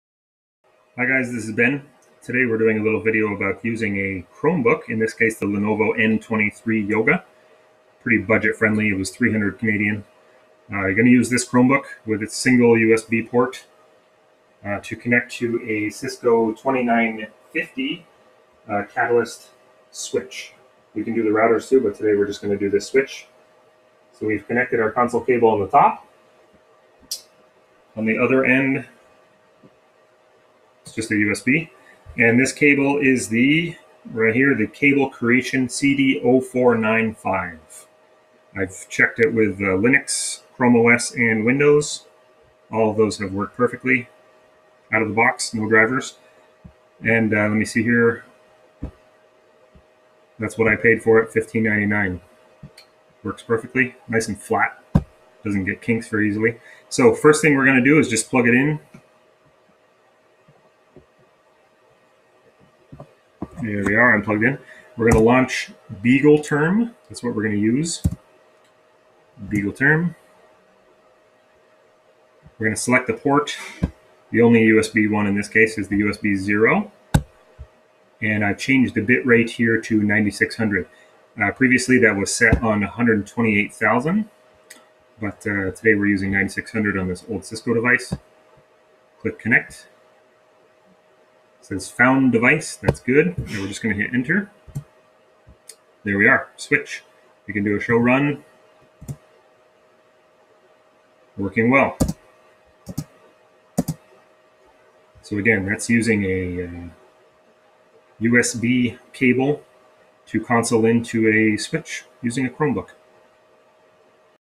Connect to Cisco Switch/Router console with a Chromebook
See how to connect to a console port on a Cisco device using a Chromebook and Beagle Term.

CableCreation console cable